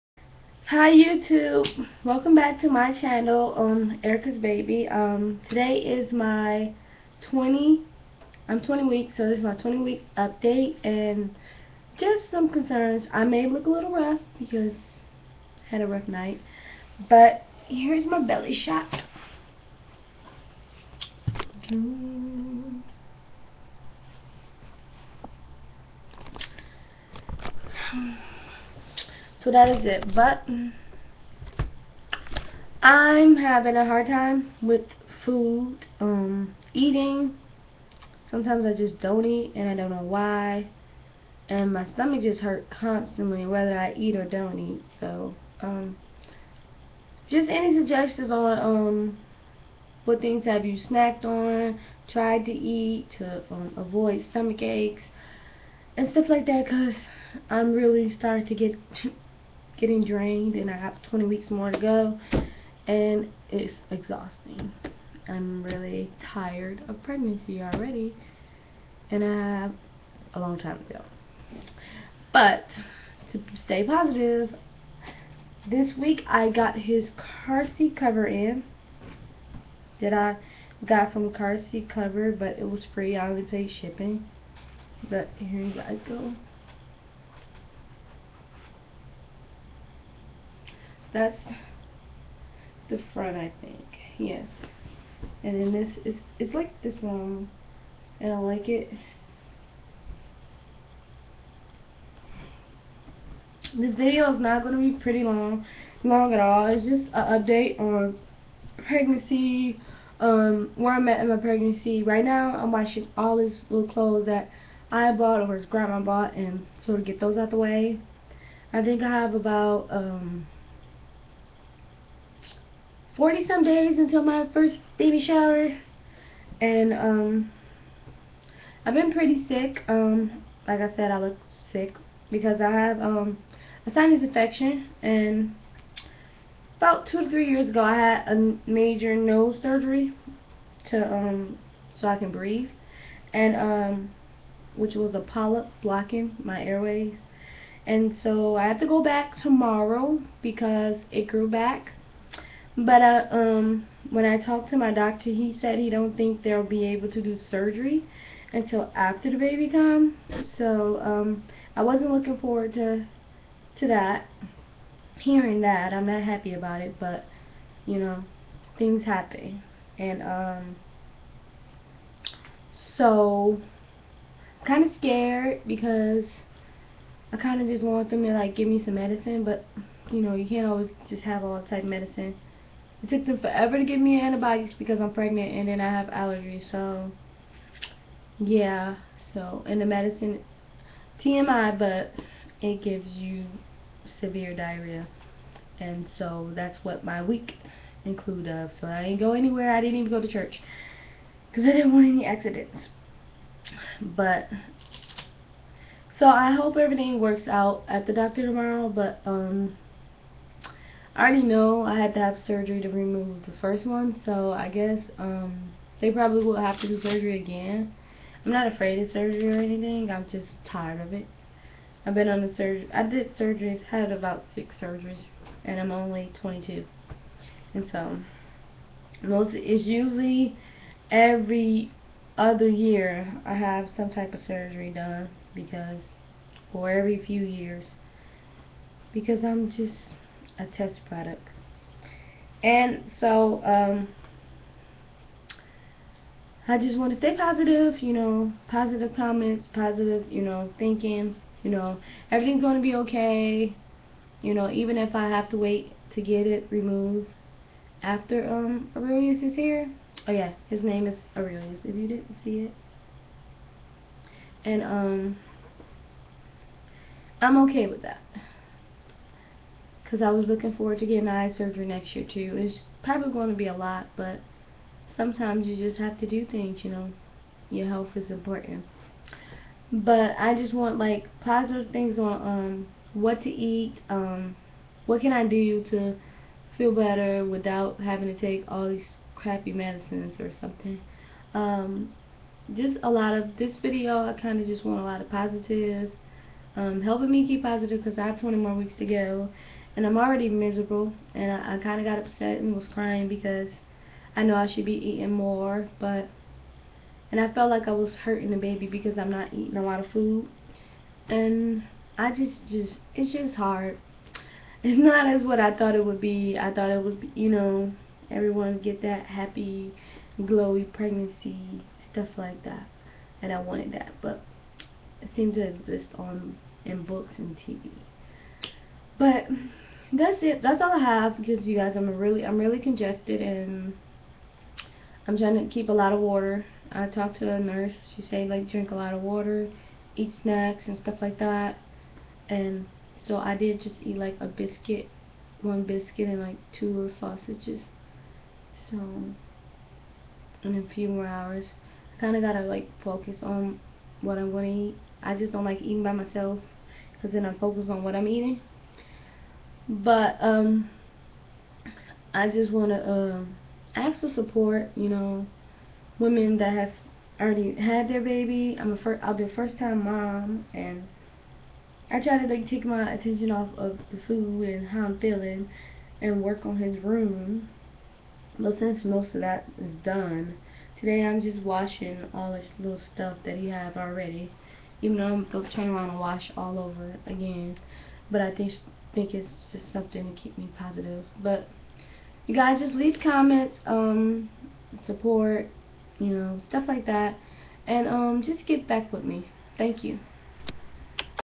Erica's baby 20 week update
showing off my 20 week belly, looking for help trying to stay positive and just a little help...Thank you my YouTube family